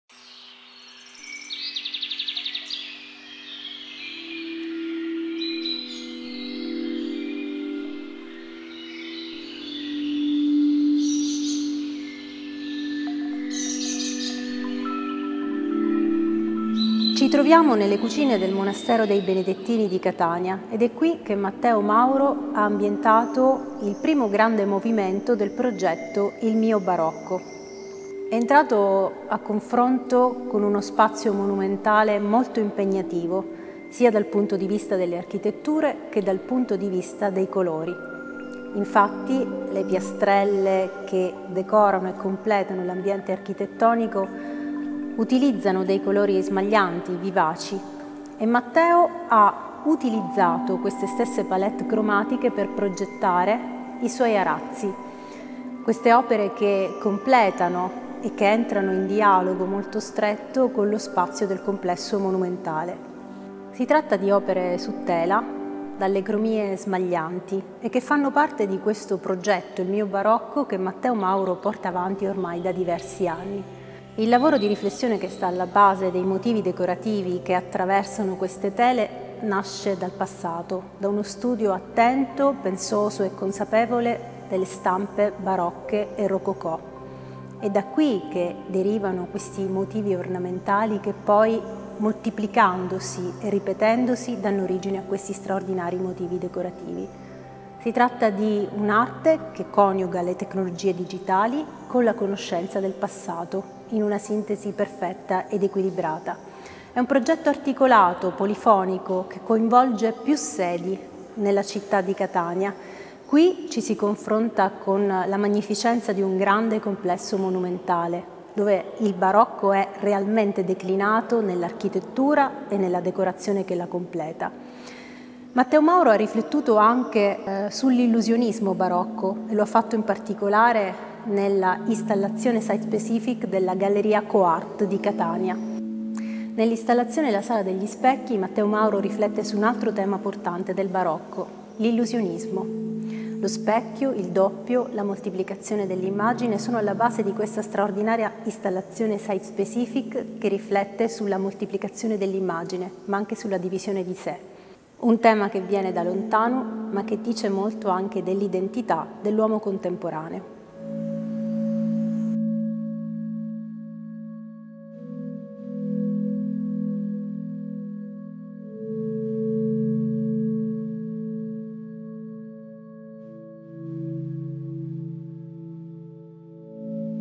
Daniela Vasta presenta Matteo Mauro - IL MIO BAROCCO- Monastero dei Benedettini Museo della Fabbrica
Catania - Dal 31 maggio al 20 giugno 2019 negli spazi dell’ex Monastero dei Benedettini,  si è svolta la mostra "Il Mio Barocco" a cura di Federica Santagati e Daniela Vasta, entrambe docenti del Dipartimento di Scienze Umanistiche dell’Università degli Studi di Catania. Il progetto, che l’artista Matteo Mauro, catanese di nascita e londinese d’adozione, sviluppa ormai da diversi anni, insegue con tenacia una sintesi fra tradizione figurativa e tecnologie digitali. La ricerca di Matteo Mauro mira alla comprensione del significato del termine micromegalico nel campo della storia dell’arte, in particolare, nell’ornamentazione Barocca e Rococò.